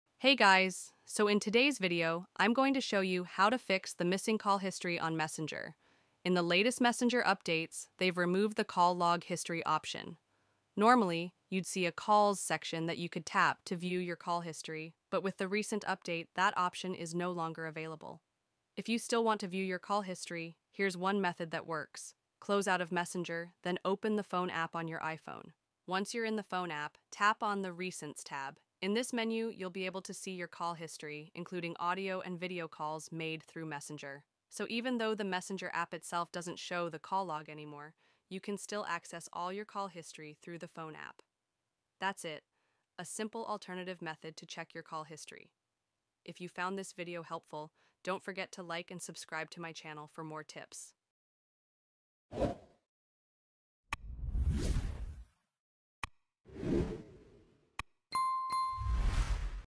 Fix Missing Call History on Messenger | 2025
Are your Messenger calls not showing up in your call history? In this 2025 updated guide, I’ll show you the quick and easy steps to fix the missing call history issue on Facebook Messenger. Whether you’re using Android or iPhone, this video will help you restore your call logs and make sure everything syncs properly.

In this video:
 • How to fix missing call history on Messenger
 • Update settings to sync calls
 • Check app permissions and privacy settings
 • Clear cache and reinstall if necessary